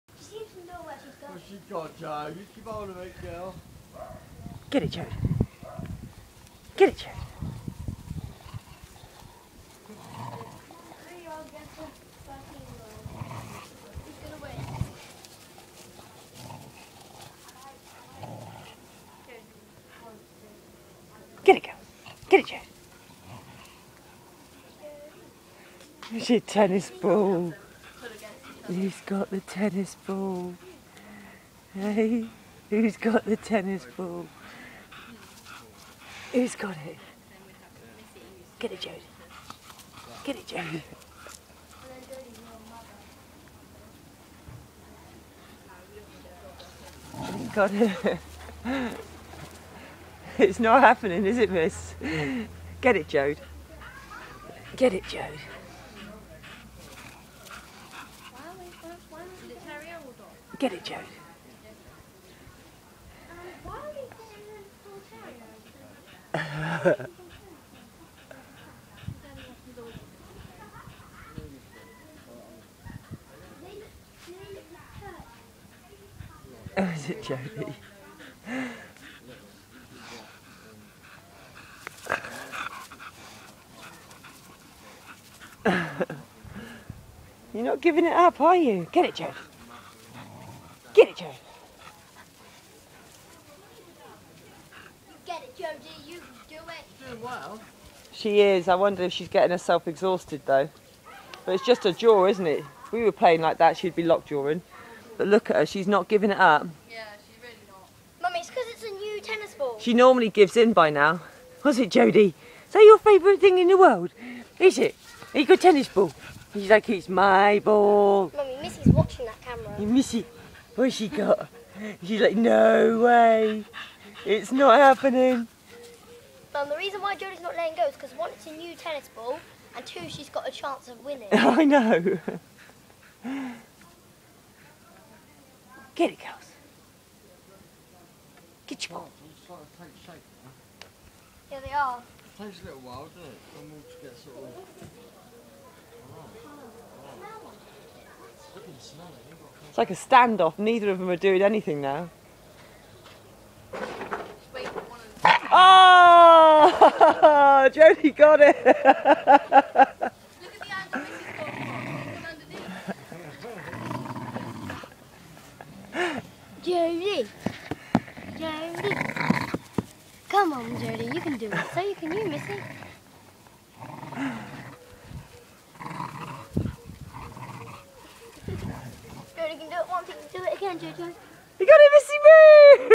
My Staffies Tug-of-war
My 13 and 4 yr old Staffies determined each one is getting the ball! Normally Jody, my old girl, gives up ... But not easily this time! :-)